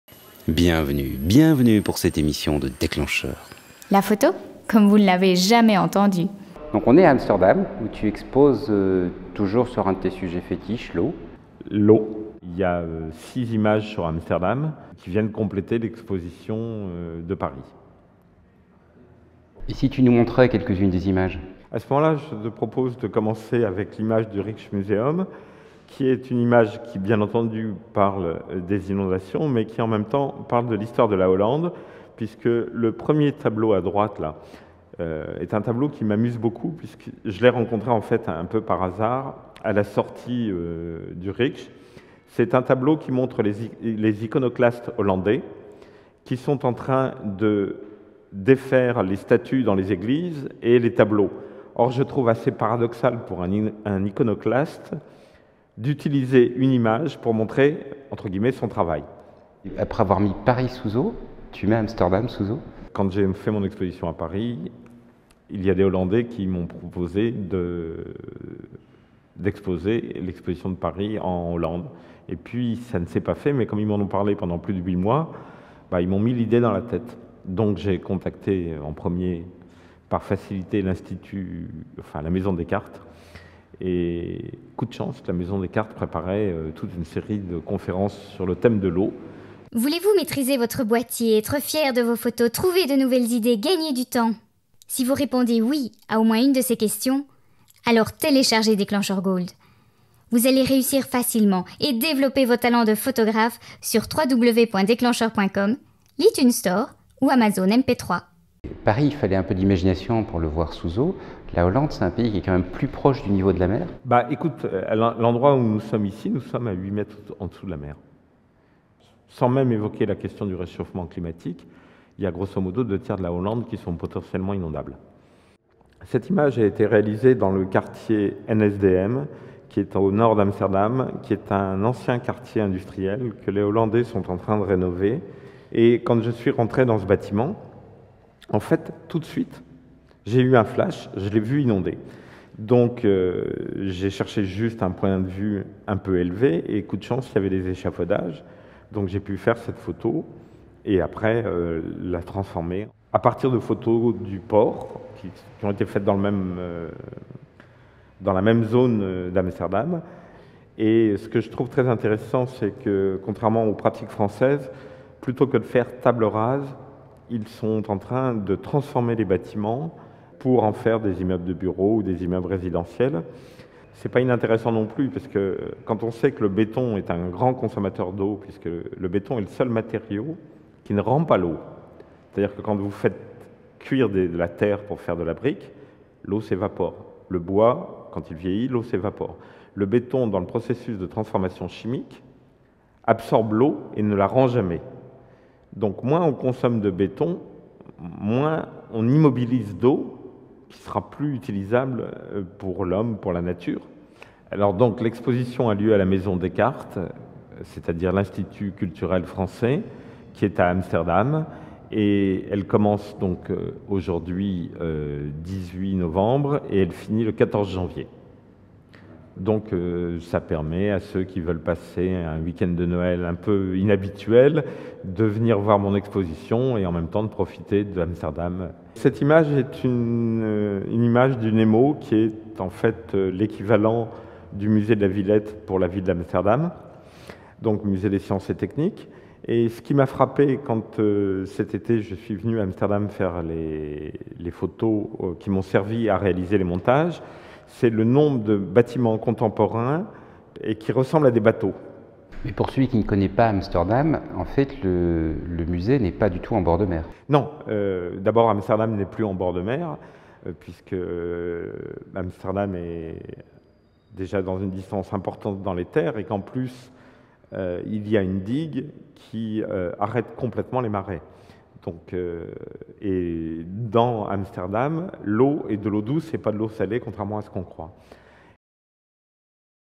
Vivre avec l'eau
Exposition "Leven met water" à la maison Descartes (Institut Français aux Pays-Bas).